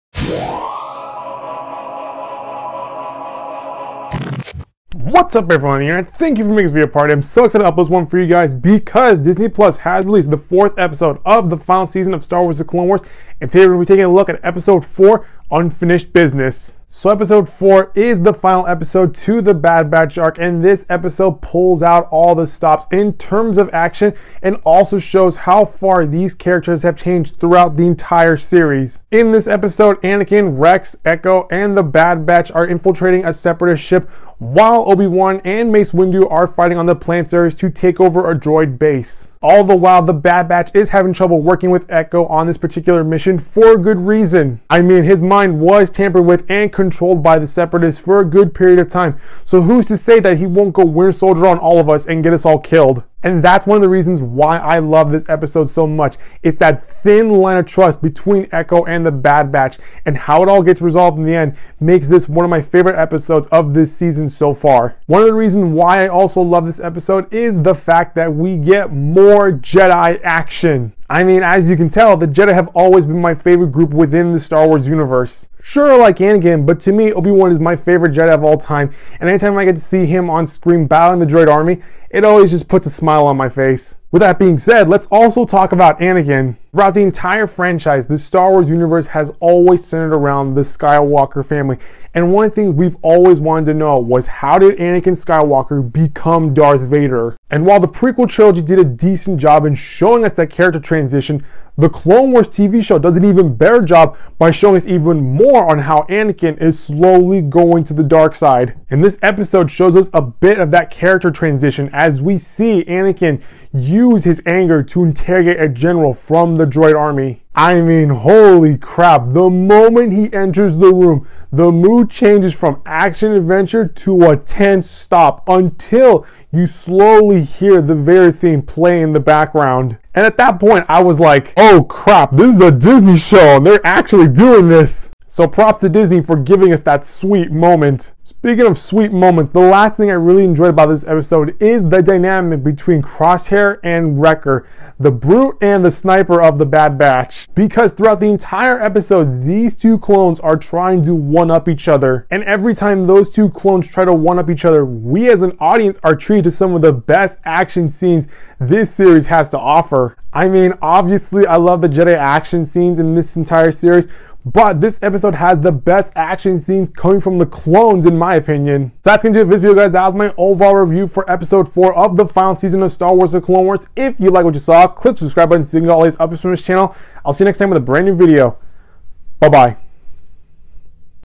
Star Wars The Clone Wars Season 7 Episode 4: Unfinished Business (Review)
What's up everyone on the internet? Thank you for making this video a part of your day! I am so excited because Disney+ has released the fourth episode of the final season of Star Wars The Clone Wars and today we're gonna be taking a look at Episode 4: Unfinished Business.